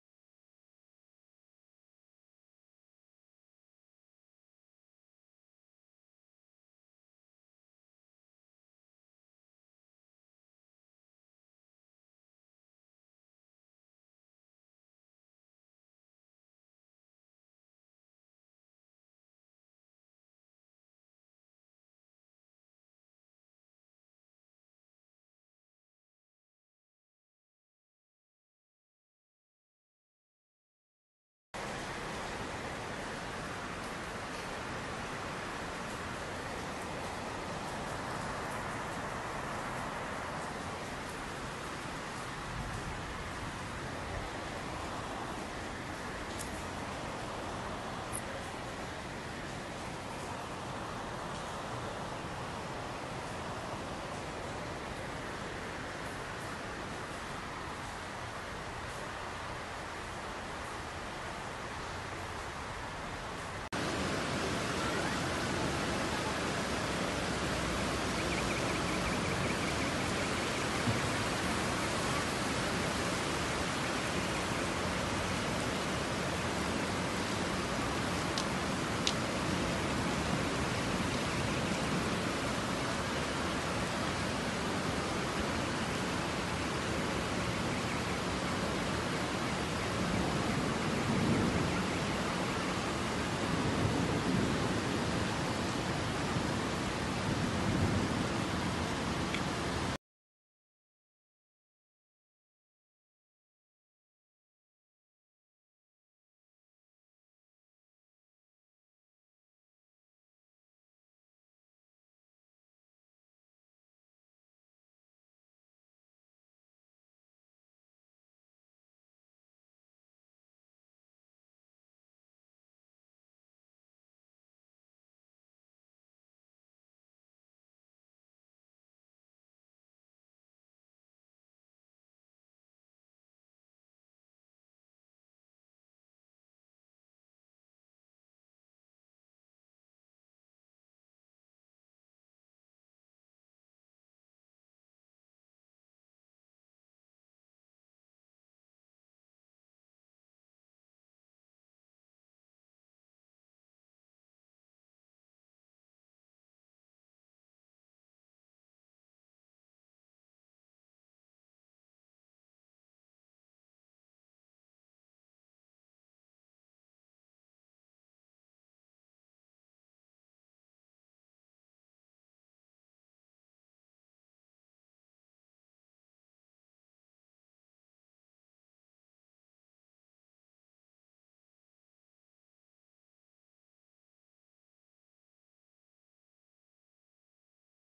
Forte chuva desabou em Campo Grande na tarde desta quarta-feira (3/10)
O temporal que desabou em Campo Grande por volta das 14h trouxe inúmeros prejuízos e perigo real aos motoristas que estavam transitando pelas ruas e avenidas da cidade.
A situação de enchente com carros quase que arrastados pelas águas mais uma vez se repetiu na avenida Nelly Martins, o temporal também castigou bairros mais afastados como, Nova Lima, Coophasul e Santa Luzia.